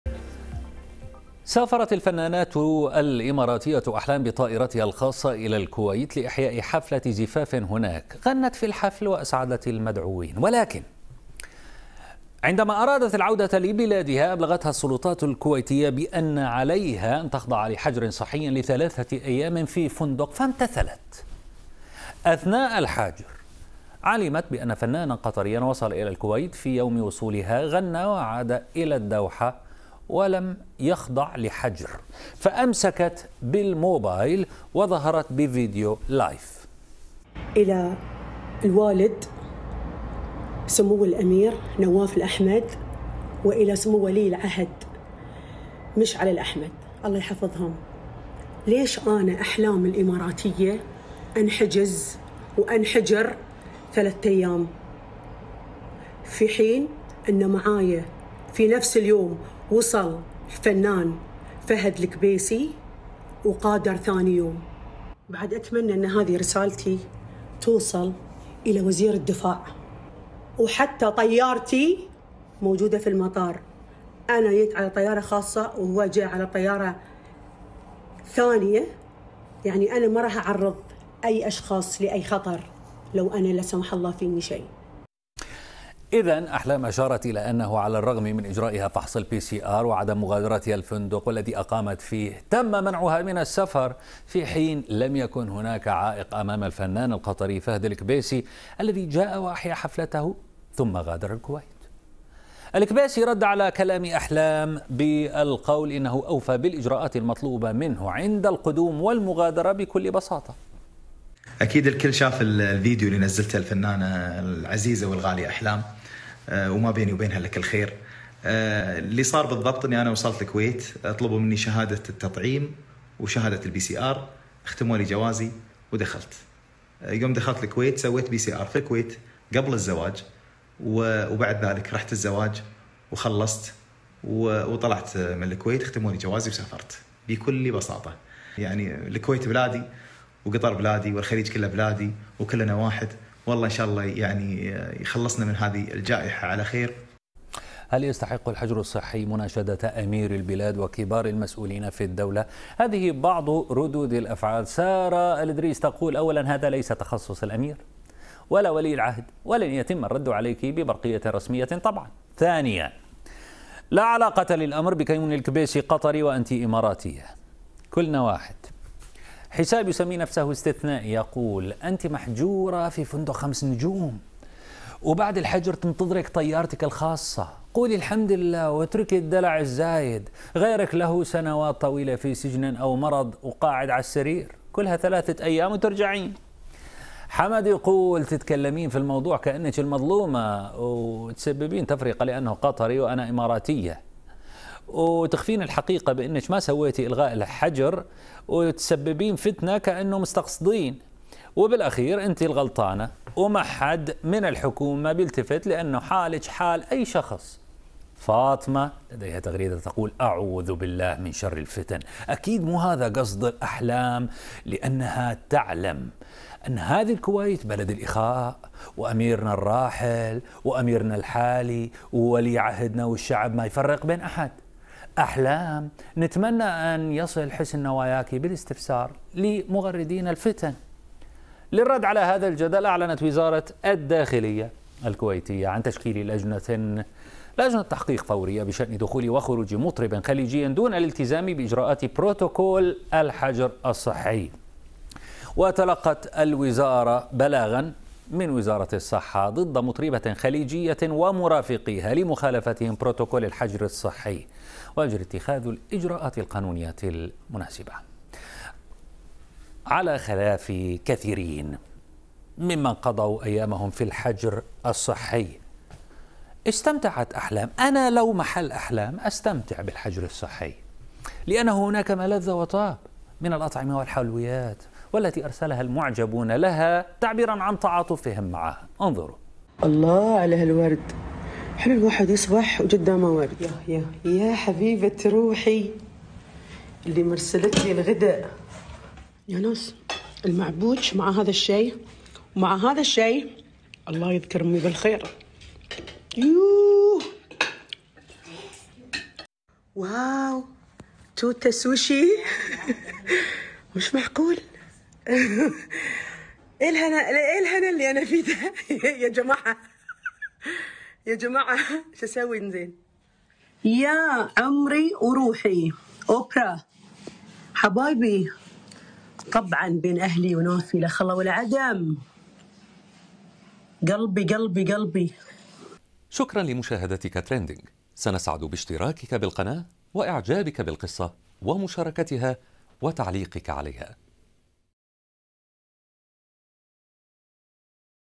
الفنانة الإماراتية أحلام تستغيث بأميرالكويت بسبب إجراءات الحجر الصحي
أحلام تستغيث بأمير الكويت بسبب الحجر الصحي.
الفنانة الإماراتية أحلام تستغيث بأمير الكويت بسبب إجراءات الحجر الصحي.